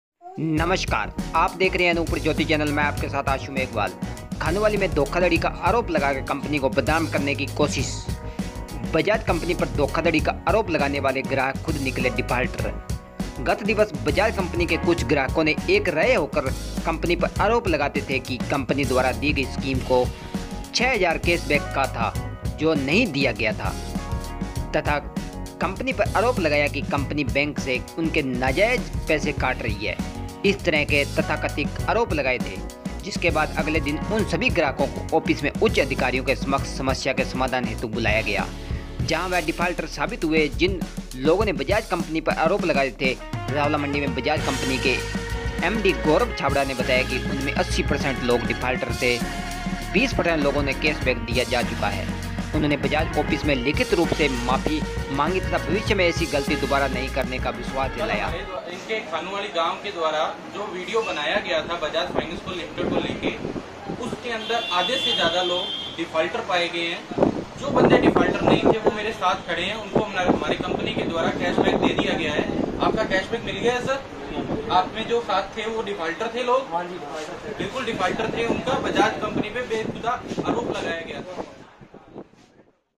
खानुवाली मे बजाज कंपनी पर धोखाधड़ी का आरोप लगाने वाले ग्राहक खुद निकले डिफाल्टर
खानुवाली मे धोखाधड़ी का आरोप लगाकर कंपनी को बदनाम करने की कोशिश
बजाज कंपनी पर धोखाधड़ी का आरोप लगाने वाले ग्राहक खुद निकले डिफाल्टर
गत दिवस बजाज कंपनी के कुछ ग्राहकों ने एक राय होकर कंपनी पर आरोप लगाए थे कि कंपनी द्वारा दी गई स्कीम जो कि ₹6000 कैशबैक का था जो नहीं दिया गया तथा कंपनी पर आरोप लगाया गया कि कंपनी बैंक से उनके नाजायज पैसे काट रही है इस तरह के तथाकथित आरोप लगाए थे
जिसके बाद अगले दिन उन सभी ग्राहकों को ऑफिस में उच्च अधिकारियों के समक्ष समस्या के समाधान हेतु बुलाया गया जहां वह डिफाल्टर साबित हुए जिन लोगों ने बजाज कंपनी पर आरोप लगाए थे  रावला मंडी में बजाज कंपनी के। एम डी गौरव छाबड़ा ने बताया कि  उनमें 80% लोग डिफाल्टर थे 20% लोगों को कैशबैक दिया जा चुका था
उन्होंने बजाज ऑफिस में लिखित रूप से माफी मांगी तथा भविष्य में ऐसी गलती दोबारा नहीं करने का विश्वास दिलाया